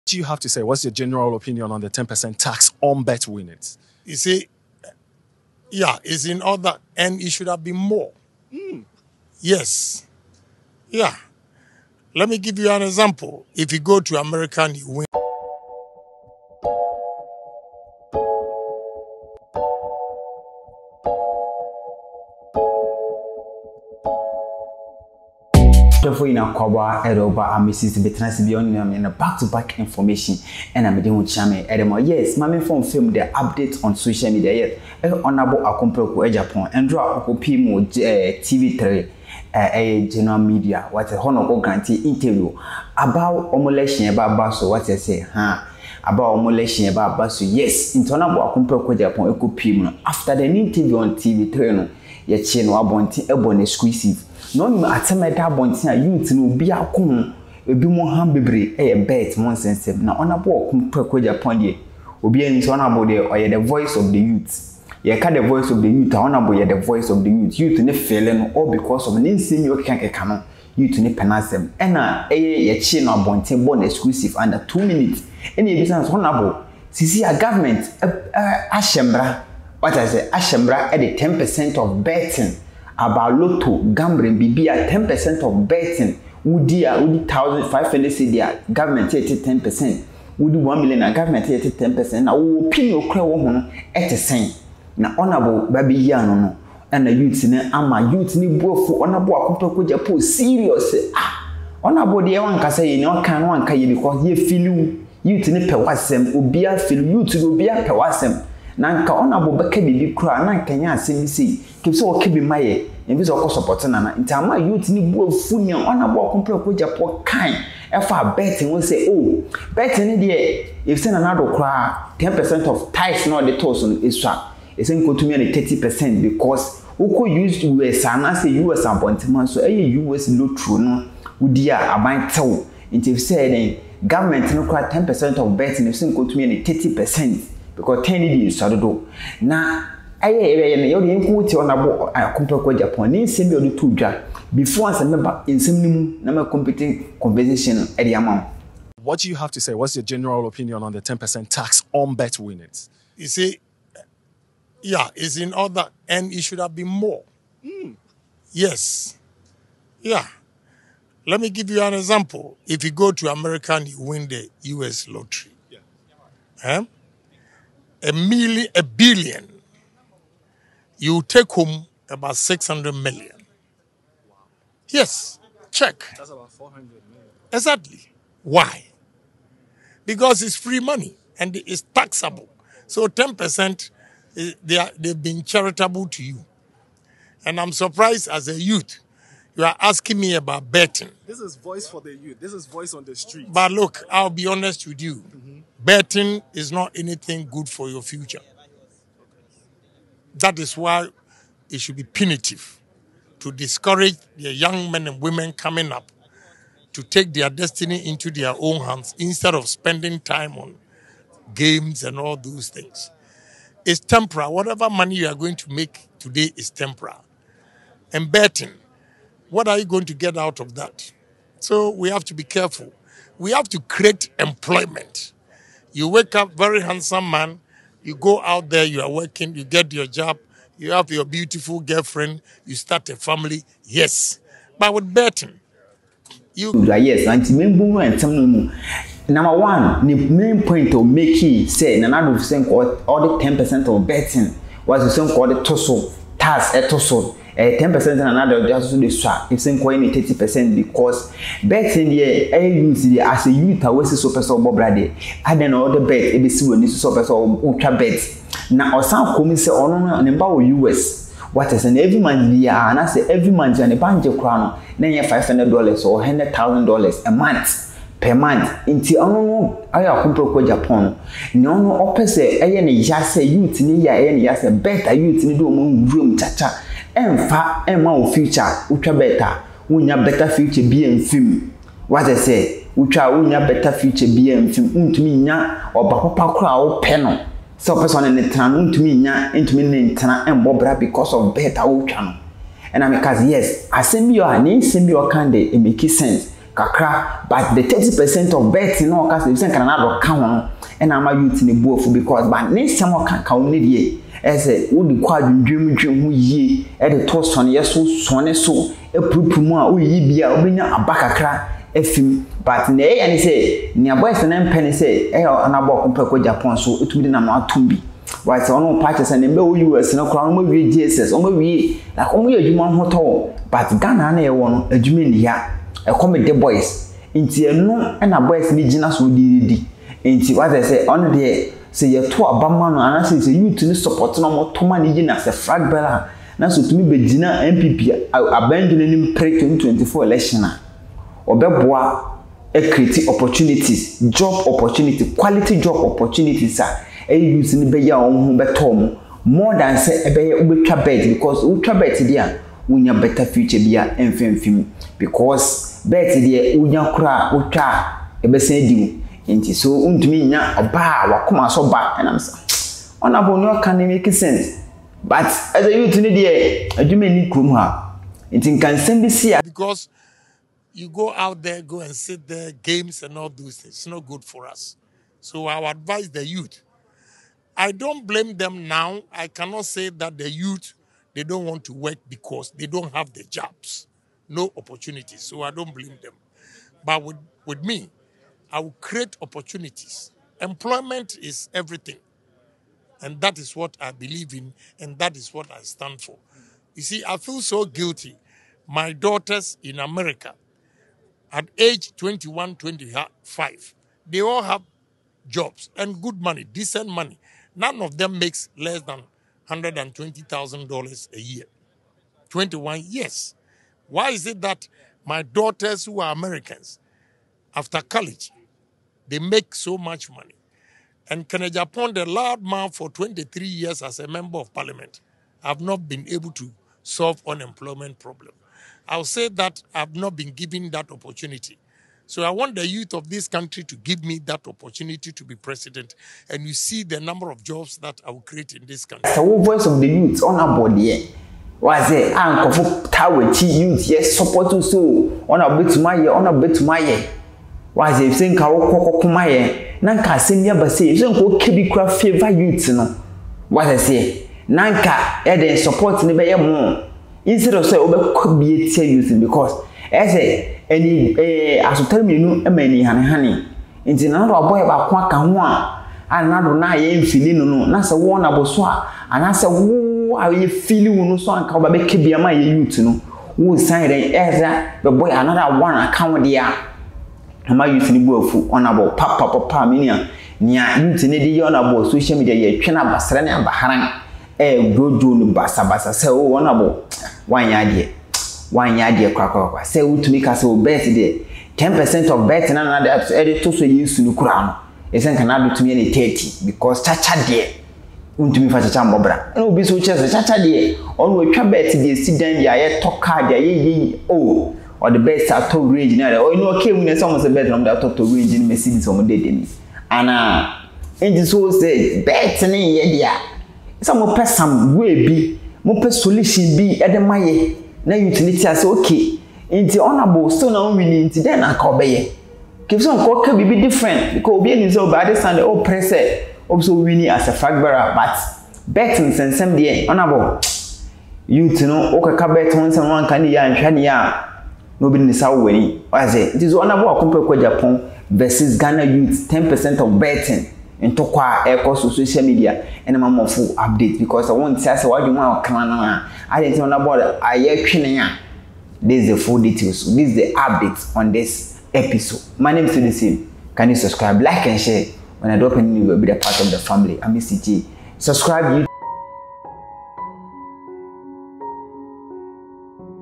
10% Tax On Bet Winning is small is should be included NPP flagbearer Kennedy Agyepong Reacts 😳😳
10% Tax On Bet Winning is small is should be included Kennedy Agyepong Reacts

AMISTY TV